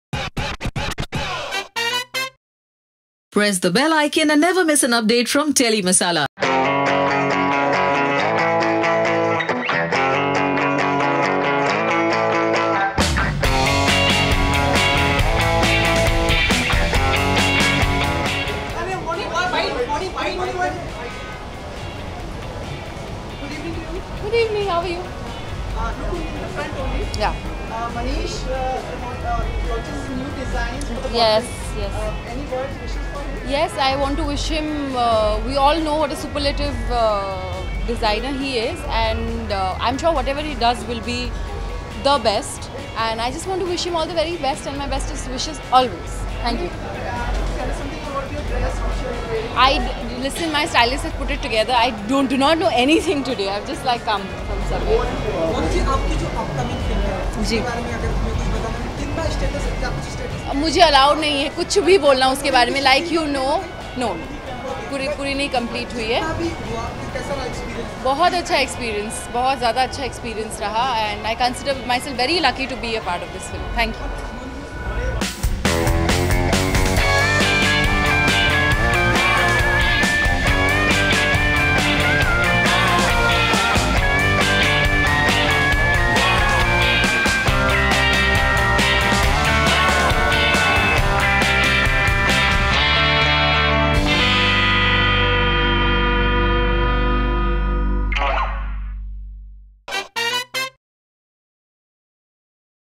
Mouni Roy SIZZLES In RED | Talks About Working With Akshay Kumar
Click on the video to have a look at the sizzling red look of Mouni Roy at an event and hear her talk about her experience of working in 'Gold' with Akshay Kumar.